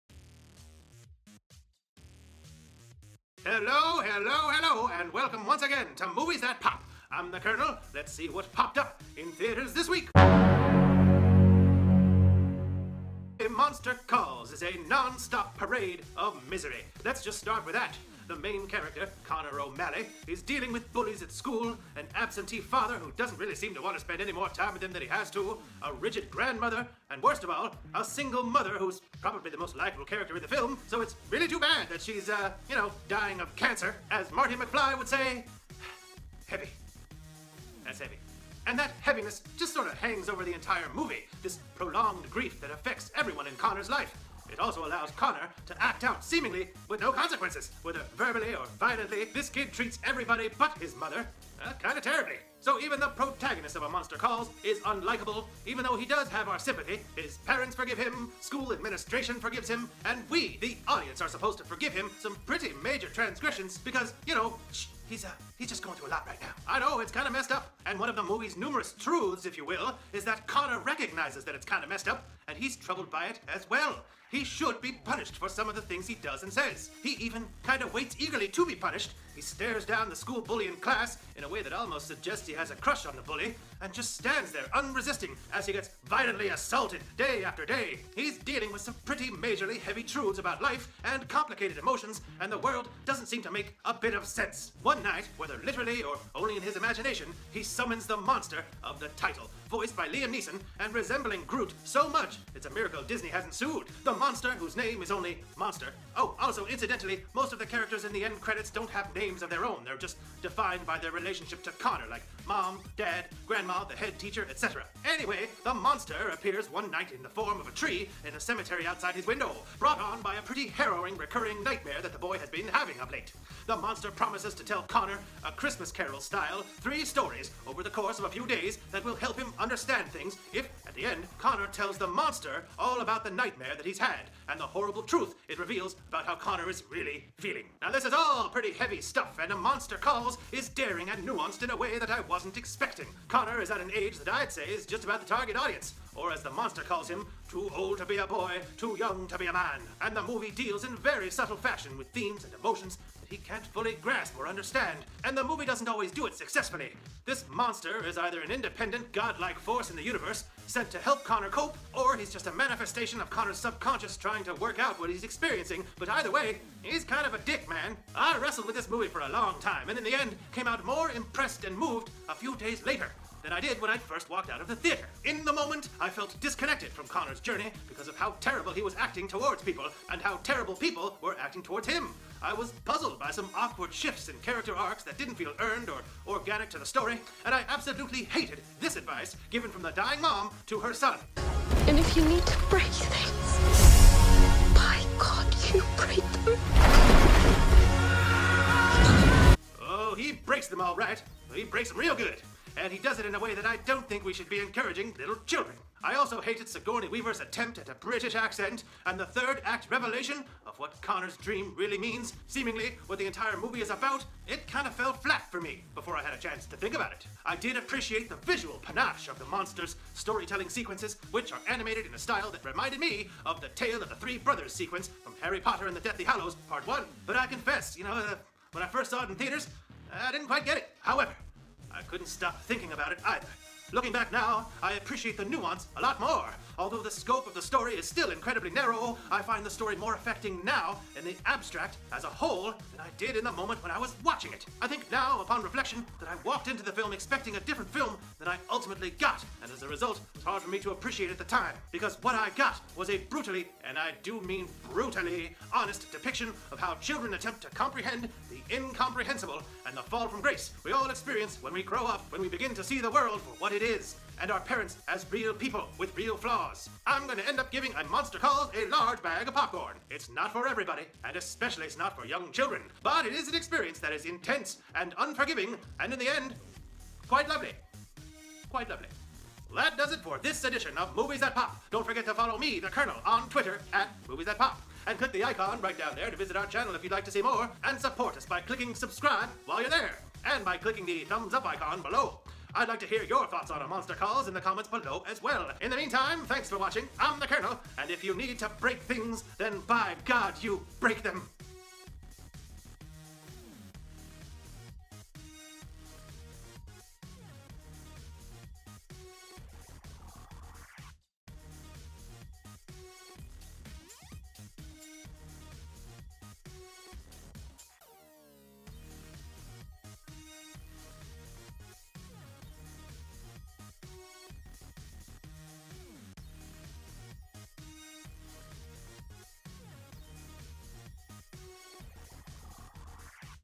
A Monster Calls Movie Review
The Kernel declares that A Monster Calls really snuck up on him, in his latest film review.